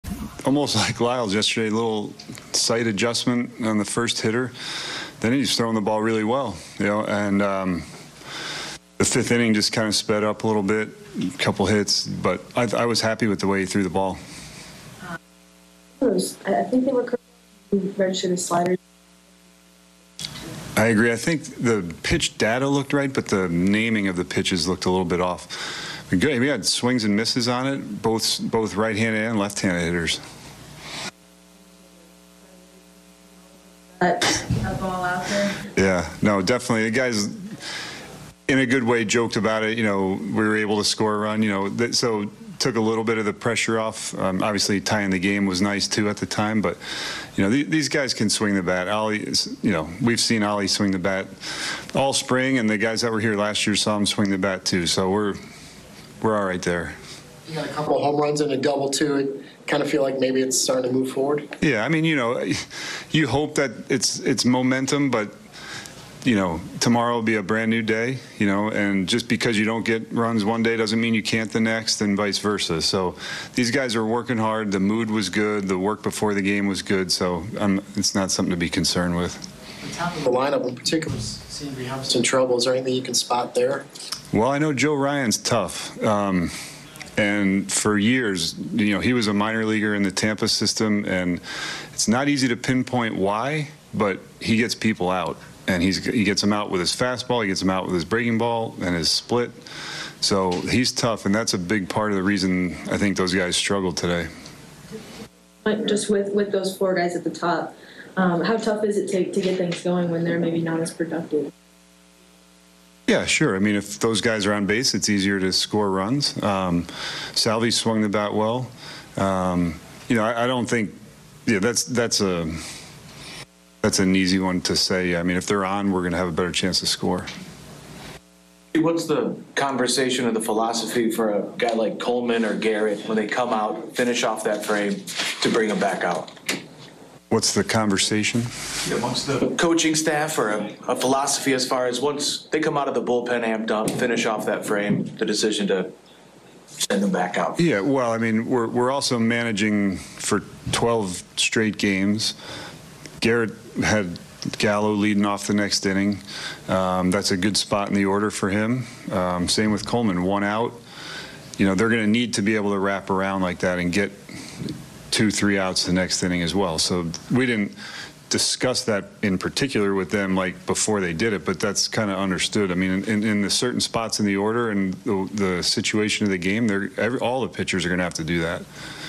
Quatraro: 'Guys are working hard' despite slow start
Matt Quatraro after a quiet opening series from the Kansas City Royals' bats: "Just because you don't get runs one day doesn't mean you can't the next and vice versa. These guys are working hard, the mood was good, the work before the game was good, so it's not something to be concerned with."